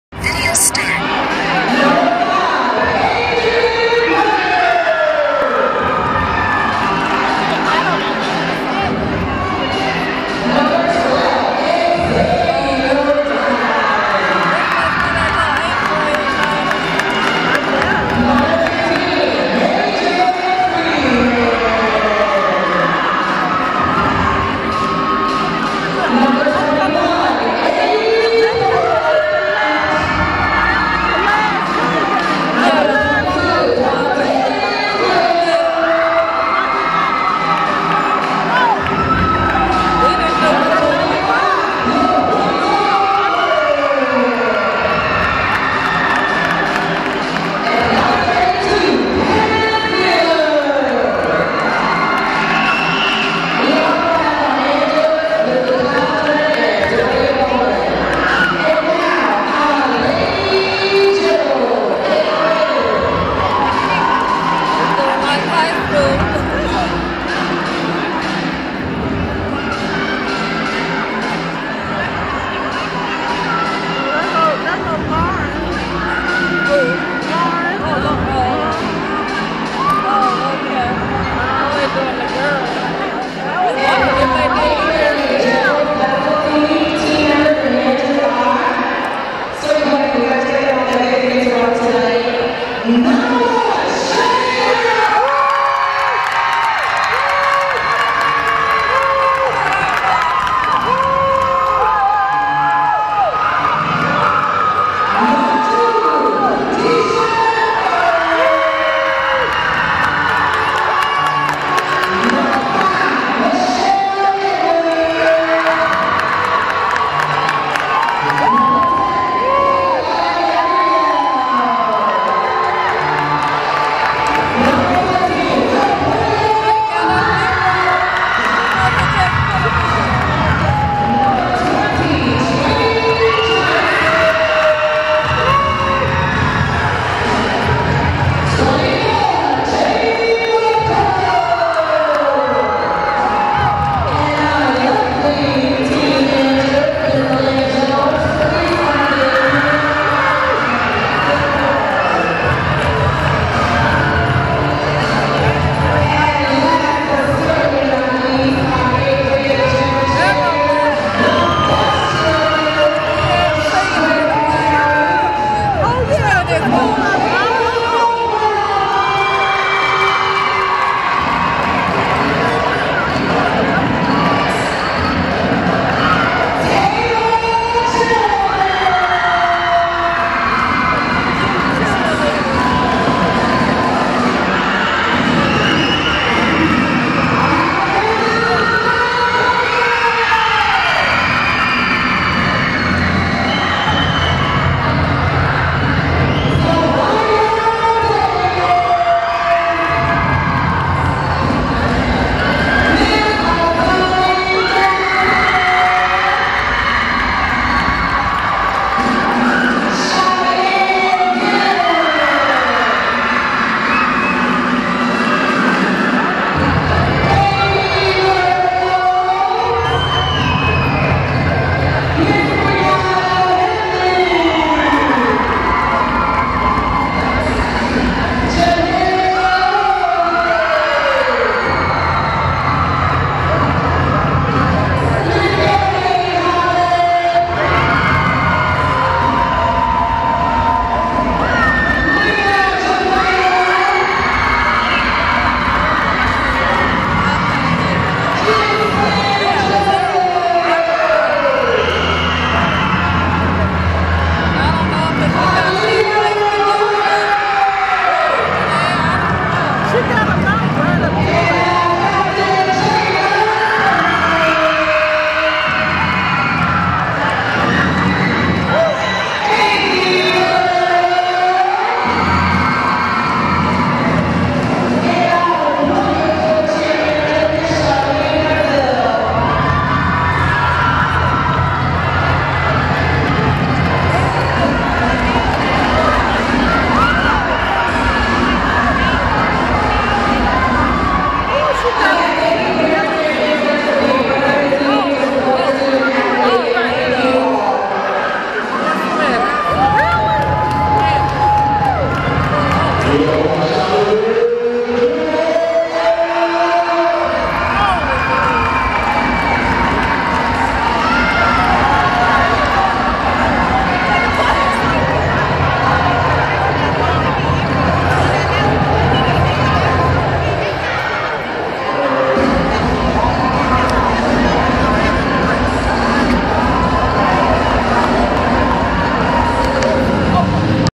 Coronation of 8th Graders Shiloh Middle School 2018 0125 182528 011
Coronation of 8th Graders Shiloh Middle School at Shiloh High School in Snellville GA